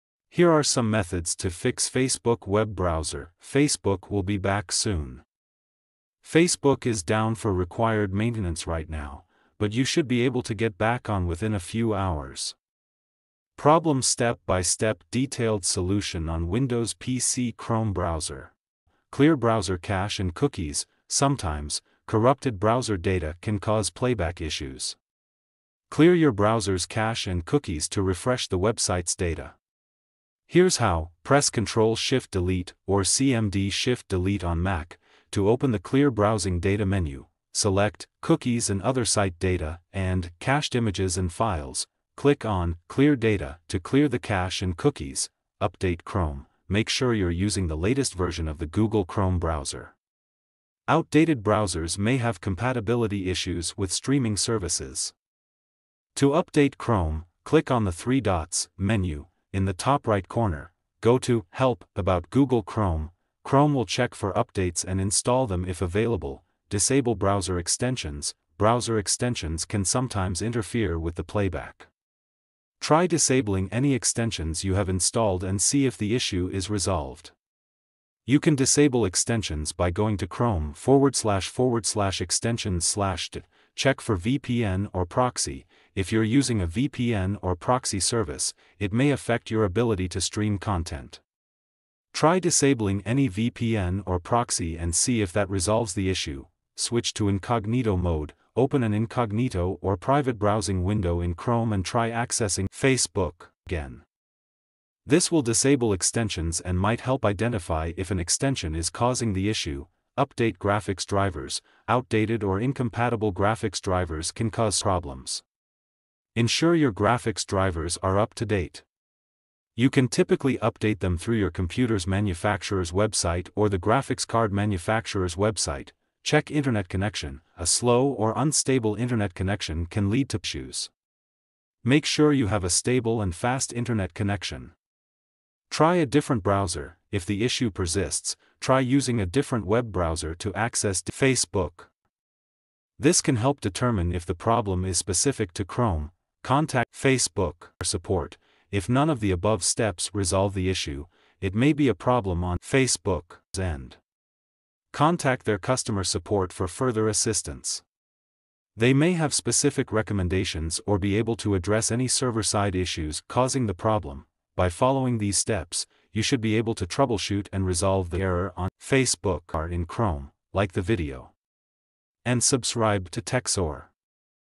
How To Fix Facebook Will Be Back Soon Facebook is down for required maintenance right now Chrome PC
How To Fix Facebook Will Be Back Soon Facebook is down for required maintenance right now, but you should be able to get back on within a few hours. Error on Windows PC Chrome Browser Problem Solved

Here are some steps you can try to resolve this issue:

Clear Browser Cache and Cookies:

Sometimes, corrupted browser data can cause playback issues. Clear your browser's cache and cookies to refresh the website's data. Here's how:
Press Ctrl + Shift + Delete (or Cmd + Shift + Delete on Mac) to open the clear browsing data menu.
Select "Cookies and other site data" and "Cached images and files."
Click on "Clear data" to clear the cache and cookies.
Update Chrome:

Make sure you're using the latest version of the Google Chrome browser. Outdated browsers may have compatibility issues with streaming services. To update Chrome:
Click on the three dots (menu) in the top-right corner.
Go to "Help" then click on "About Google Chrome."
Chrome will check for updates and install them if available.
Disable Browser Extensions:

Browser extensions can sometimes interfere with the playback. Try disabling any extensions you have installed and see if the issue is resolved. You can disable extensions by going to chrome://extensions/.
Check for VPN or Proxy:

If you're using a VPN or proxy service, it may affect your ability to stream content. Try disabling any VPN or proxy and see if that resolves the issue.
Switch to Incognito Mode:

Open an incognito or private browsing window in Chrome and try accessing Facebook again. This will disable extensions and might help identify if an extension is causing the issue.
Update Graphics Drivers:

Outdated or incompatible graphics drivers can cause streaming problems. Ensure your graphics drivers are up to date. You can typically update them through your computer's manufacturer's website or the graphics card manufacturer's website.
Check Internet Connection:

A slow or unstable internet connection can lead to playback issues. Make sure you have a stable and fast internet connection.
Try a Different Browser:

If the issue persists, try using a different web browser to access Facebook. This can help determine if the problem is specific to Chrome.
Contact Facebook Support:

If none of the above steps resolve the issue, it may be a problem on Facebook's end. Contact their customer support for further assistance. They may have specific recommendations or be able to address any server-side issues causing the problem.
By following these steps, you should be able to troubleshoot and resolve the error on Facebook in Chrome.

Talk soon,
Abid Akay